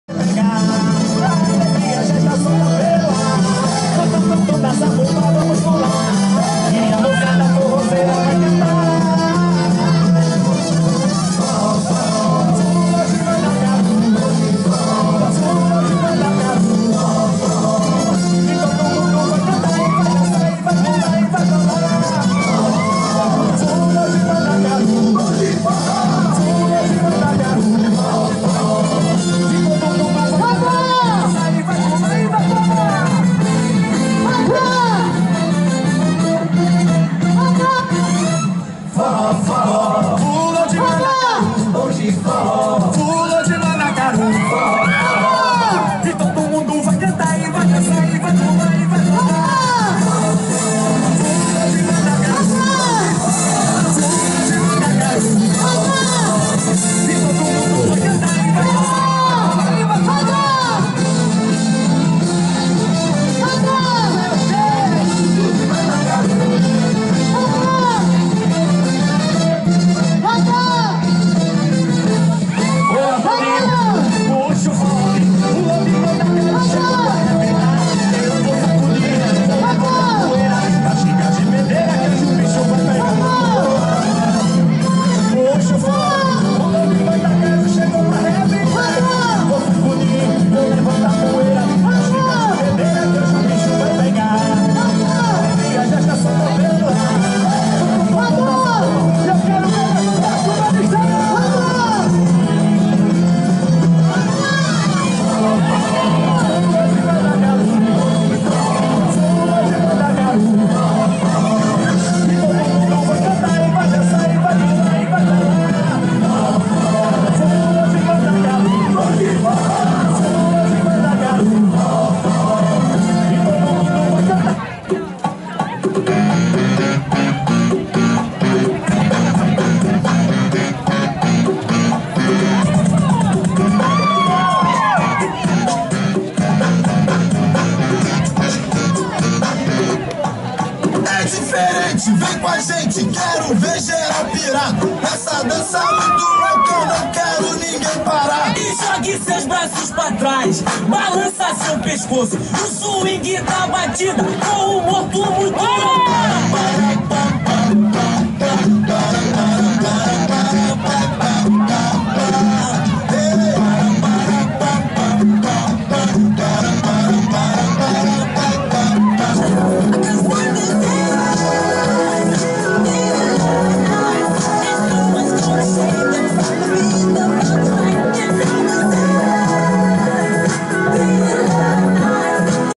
Dança da Quadrinha no Arraiá da Comunidade Nossa Senhora de Fátima
Dança da Quadrinha no Arraiá 2017 da Comunidade Nossa Senhora de Fátima de Ji-Paraná.